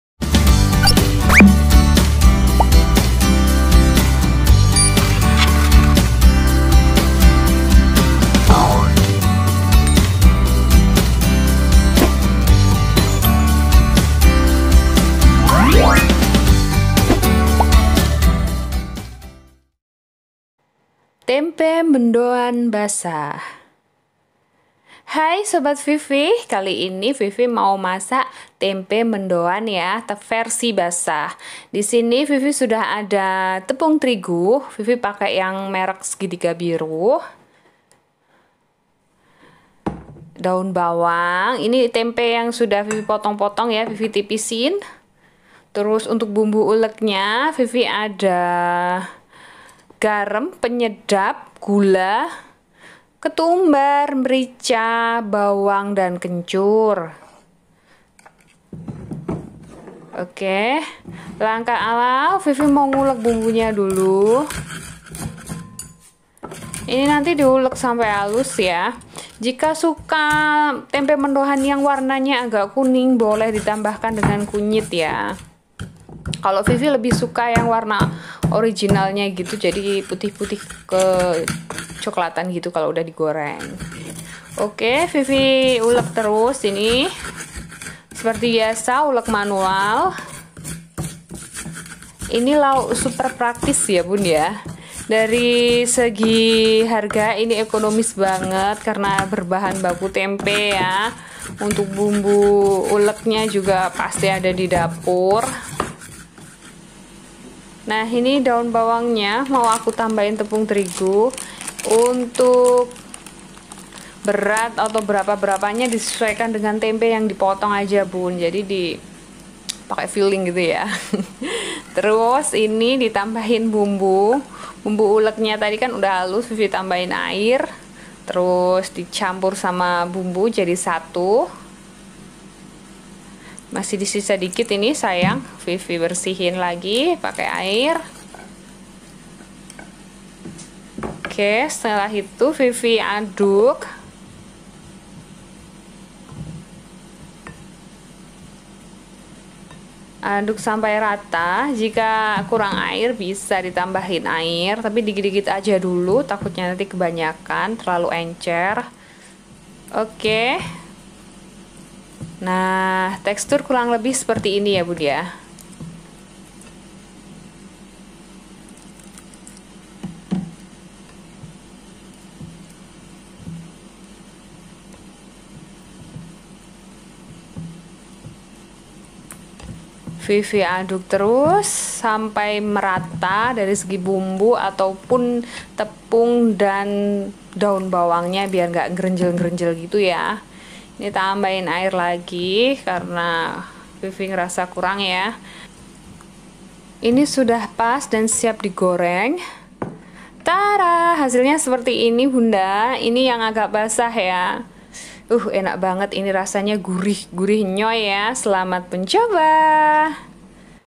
Masak tempe jadi gorengan mendoan super gurih ✅
Bumbu uleg

Garam, penyedap Gula
ketumbar
merica
Bawang Putih
kencur

bahan lain
tempe
tepung terigu
Daun bawang
Minyak Goreng